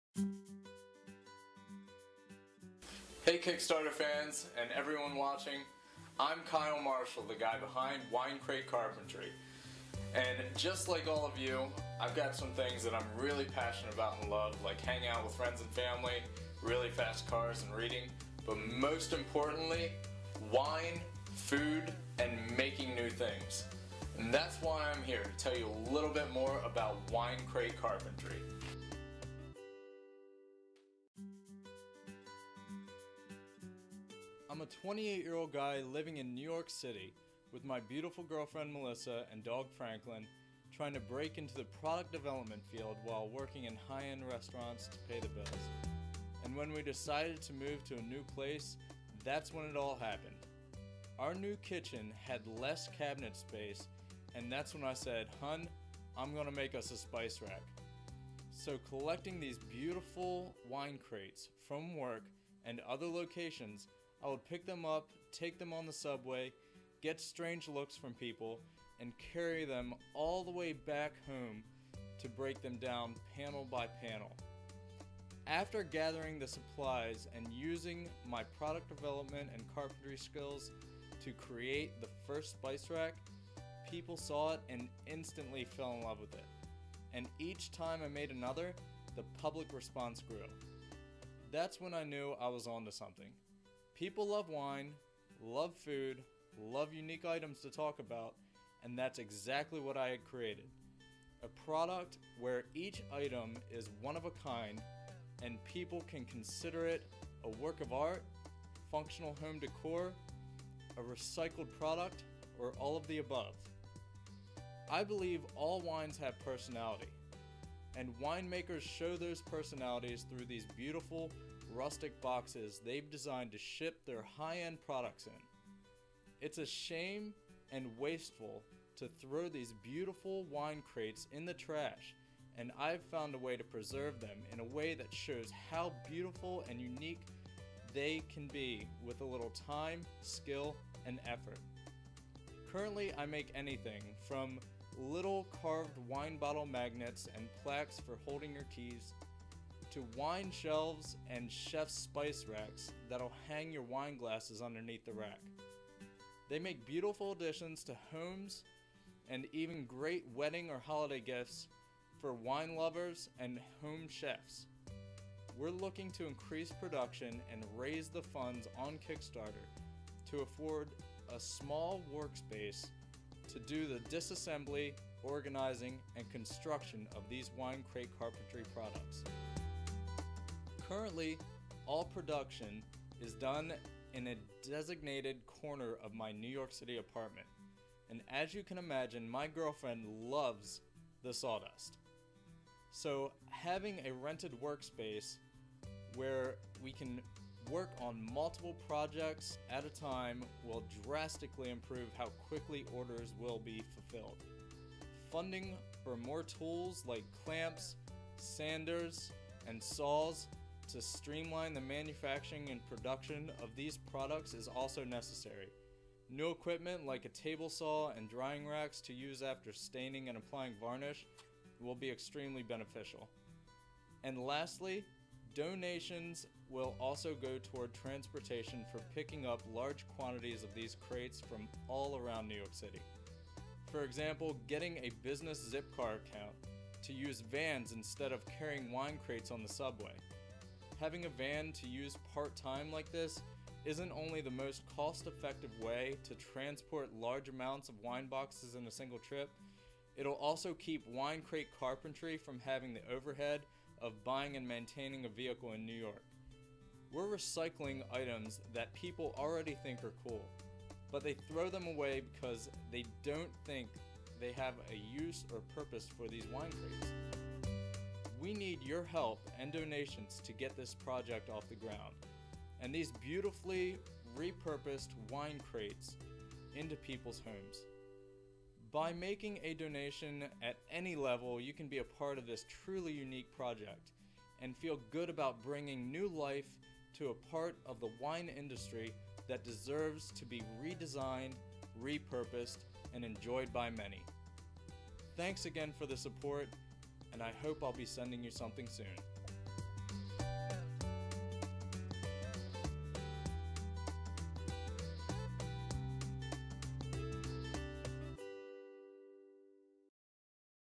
Wine Crate Carpentry
Home and Kitchen,Wine Crate Carpentry, spice racks and wine shelves

Hey Kickstarter fans and everyone watching. I'm Kyle Marshall, the guy behind Wine Crate Carpentry. Just like most people I have things I'm really passionate about and love. Like friends and family, cool cars and reading. But most of all Food, Wine, and making new things. That's why I'm here to tell you more about Wine Crate Carpentry.
I'm a 28 year old guy living in New York City, with my beautiful girlfriend Melissa and dog Franklin, trying to break into the product development field while working in high end restaurants to pay the bills. And when we decided to move to a new place that's when it all happened. Our new kitchen had less cabinet space and that's when I said, "Hun, I'm gonna make us a spice rack!". So collecting these beautiful wine crates from work and other locations, I would pick them up, take them on the subway , get strange looks from people,and carry them all the way back home to break them down panel by panel. After gathering the supplies and using my product development and carpentry skills to create the first spice rack, people saw it and instantly fell in love with it. And each time I made another, the public response grew. That's when I knew I was onto something. People Love Wine, Love Food, Love Unique Items to talk about, and that's exactly what I had created! A product where each item is one of a kind and people can consider it to be a work of art, functional home decor, a recycled product or all of the above. I believe all wines have personality and winemakers show those personalities through these beautiful rustic boxes they've designed to ship their high end products in. It's a shame and wasteful to throw these beautiful wine crates in the trash and I've found a way to preserve them in a way that shows how unique and beautiful they can be with a little time, skill and effort. Currently I make anything from little carved wine bottle magnets and Plaques for holding your keys, to Wine Shelves and Chef's spice racks that'll hang your wine glasses underneath the rack. They make beautiful additions to peoples homes, and even great wedding or holiday gifts for wine lovers and home chefs. We're looking to increase production and raise the funds on Kickstarter to afford a small work space to do the disassembly, organizing and construction of these Wine Crate Carpentry Products. Currently all production is done in a designated corner area of my New York City apartment and as you can imagine my girlfriend LOVES the sawdust. So having a rented workspace where we can work on multiple projects at time will drastically improve how quickly orders will be fulfilled.

Funding for more tools like clamps, sanders and saws to streamline the manufacturing and production of these products is also necessary. New equipment like a table saw and drying racks to use after staining and applying varnish will be extremely beneficial. And lastly donations will also go toward transportation for picking up large quantities of these wine crates from all around New York City. For example, getting a business zipcar account to use vans instead of carrying wine crates on the subway. Having a van to use part-time like this isn't only the most cost effective way to transport large amounts of wine boxes in a single trip, it'll also keep Wine Crate Carpentry from having the overhead of buying and maintaining a vehicle within New York. We're recycling items that people already think are cool, but they throw them away because they don't think they have a use or purpose for these wine crates... We need your help and donations to get this project off the ground and these beautifully repurposed Wine Crates into peoples homes. By making a donation at any level you can be a part of this truly unique project and feel good about bringing new life to a part of the wine industry that deserves to be redesigned, repurposed and enjoyed by many.
Thanks again for the support, And I hope I'll be sending something soon.